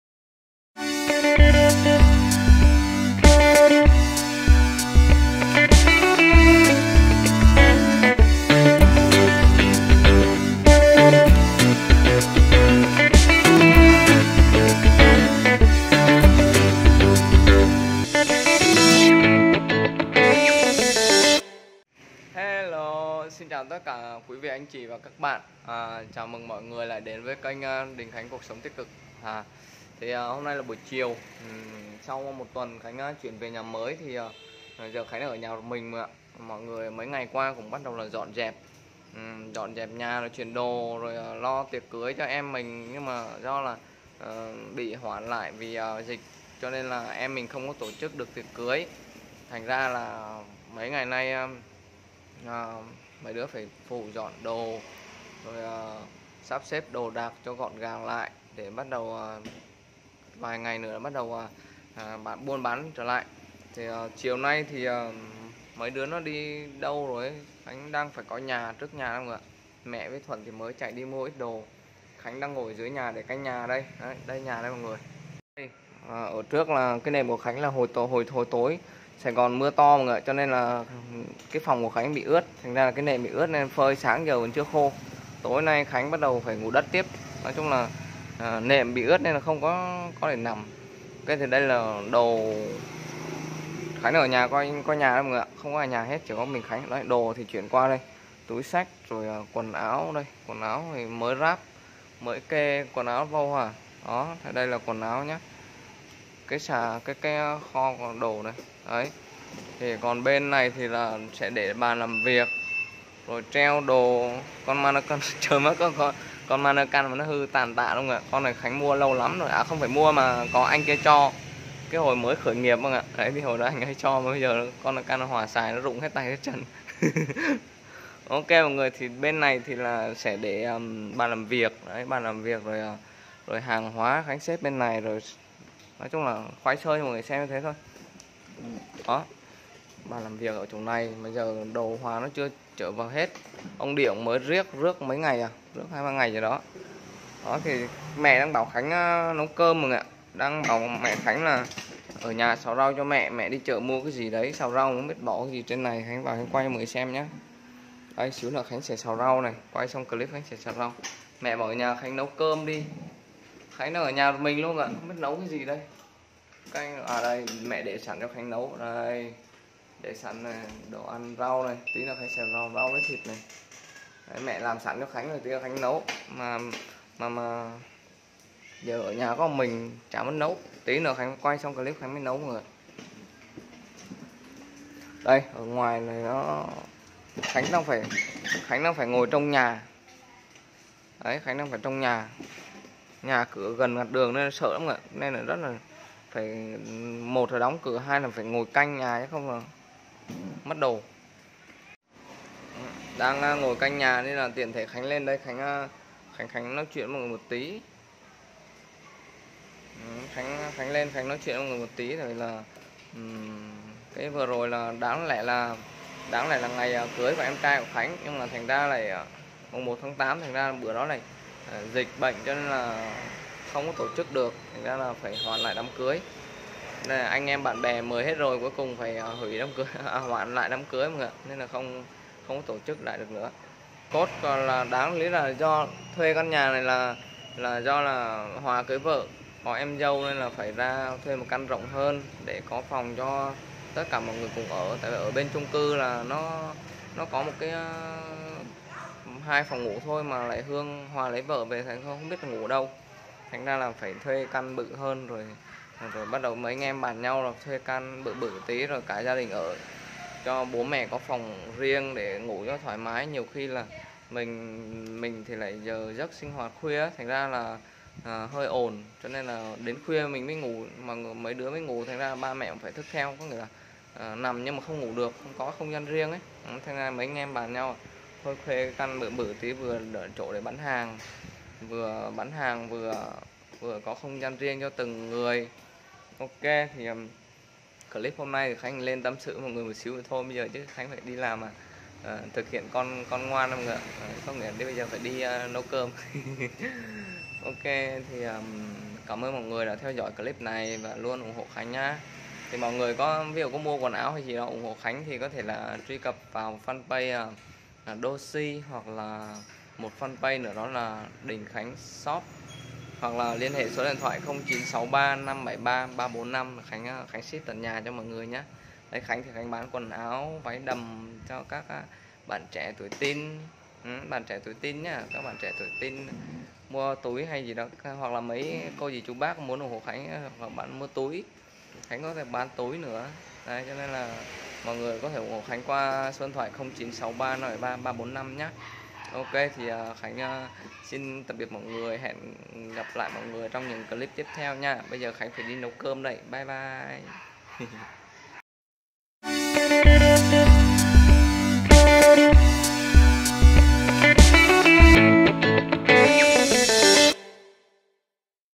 NGỒI CANH NHÀ TIỆM NÓI XÀM - ĐÌNH KHÁNH CUỘC SỐNG TÍCH CỰC
Mọi người thường bảo Khánh thường bán đồ gì thì clip này K có khoe cho mọi người xem ạ

► Theo dõi Kênh TikTok : Tô Đình Khánh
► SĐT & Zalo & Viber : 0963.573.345